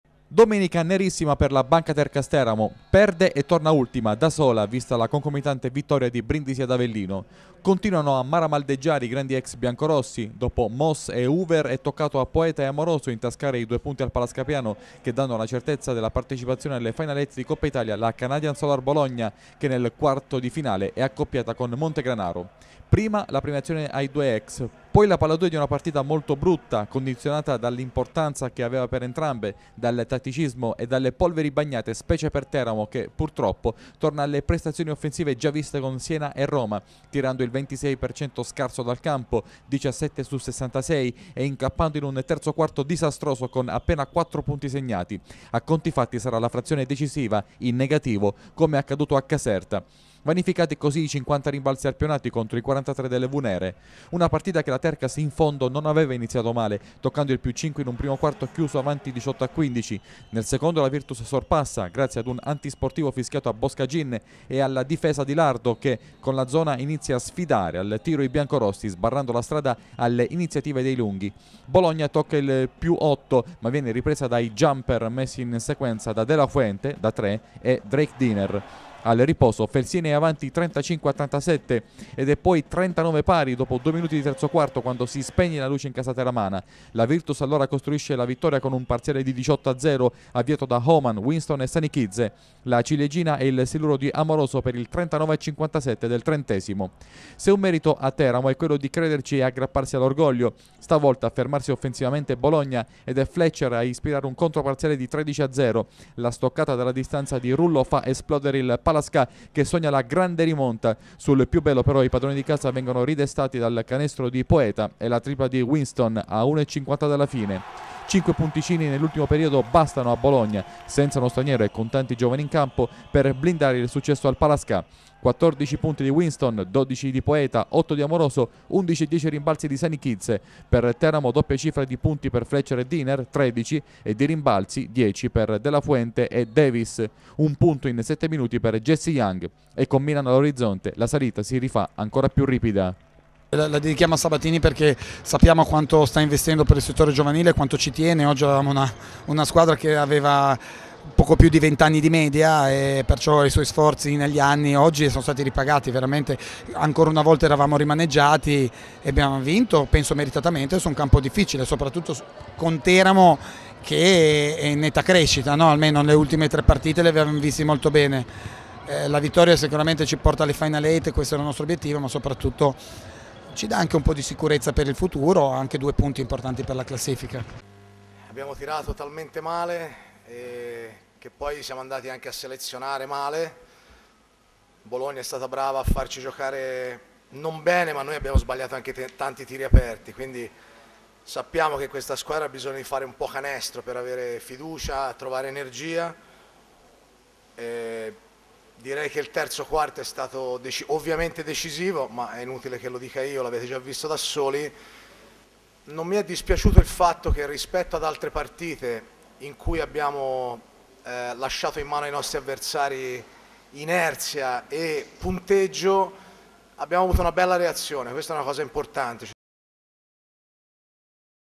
Tercas, una giornataccia. Ridono Poeta e Amoroso
Una pessima giornata al tiro costa la partita alla Banca Tercas Teramo. Ritorno al Palascapriano da vincitori per Peppe Poeta e Valerio Amoroso che volano alle final eight con la loro Canadian Solar Bologna